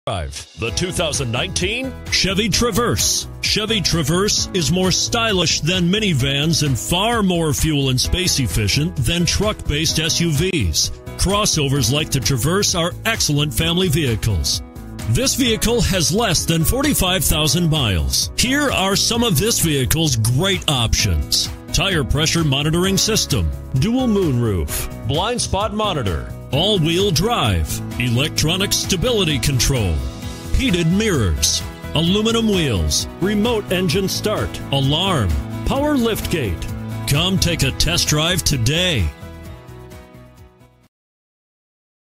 2019 Chevrolet Traverse Nanuet NY KJ153607T
2019 Chevrolet Traverse AWD 4dr Premier w/1LZ

Summit White 2019 Chevrolet Traverse Premier AWD 9-Speed Automatic 3.6L V6 SIDI VVTbrbrRecent Arrival!brbrAwards:br * 2019 KBB.com 12 Best Family Cars Some of our Pre-Owned vehicles may be subject to unrepaired safety recalls. Check for a vehicle's unrepaired recalls by VIN